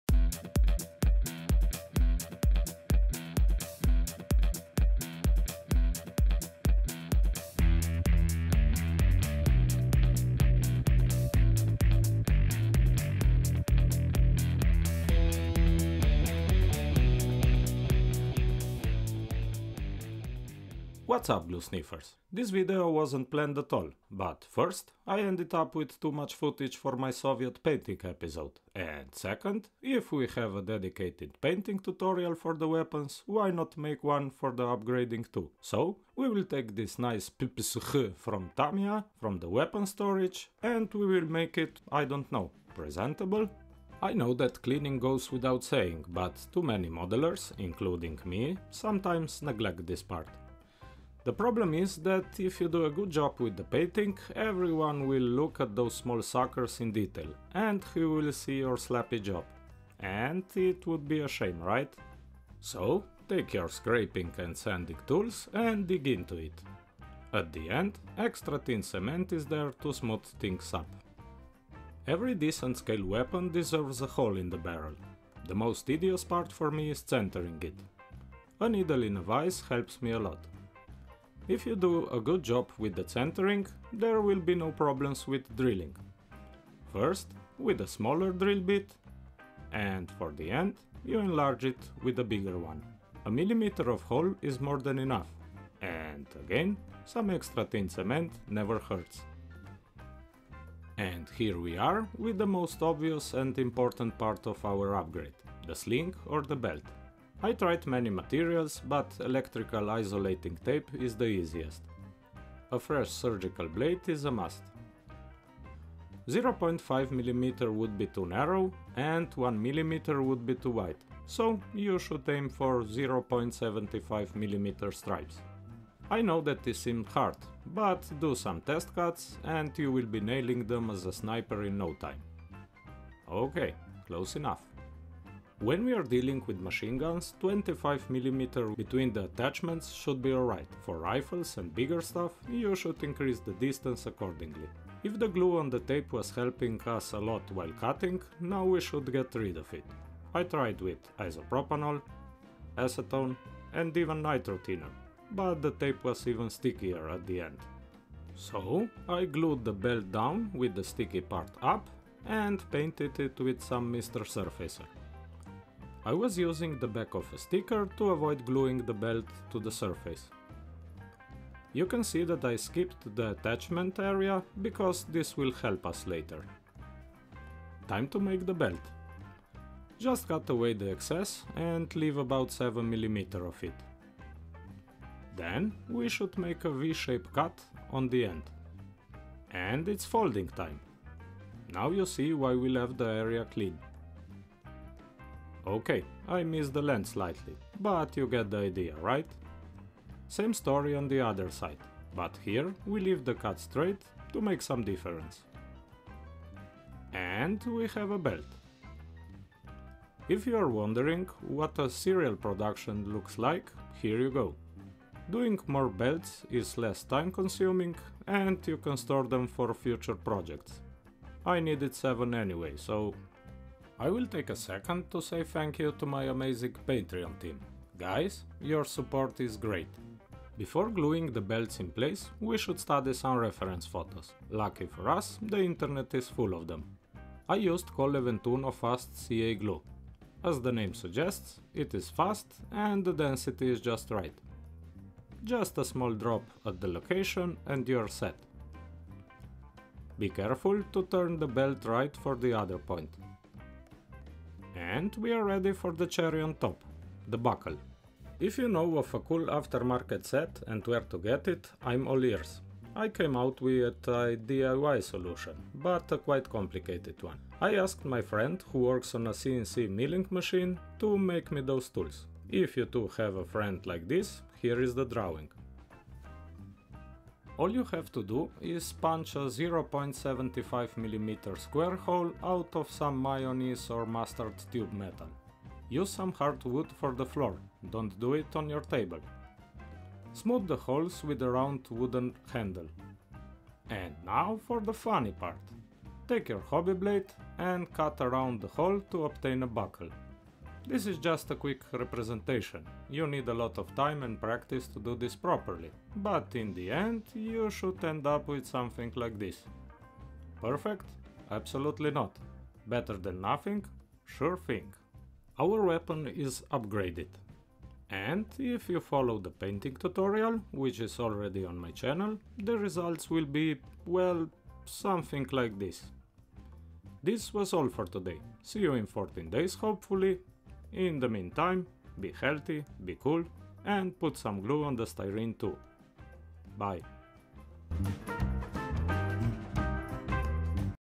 UPGRADING scale WEAPONS: Slings, Belts, Buckles (1/35, Scratchbuild)
A quick video about upgrading 1/35-scale weapons and guns to make them presentable. A step-by-step easy tutorial about making rifle slings, belts, and buckles.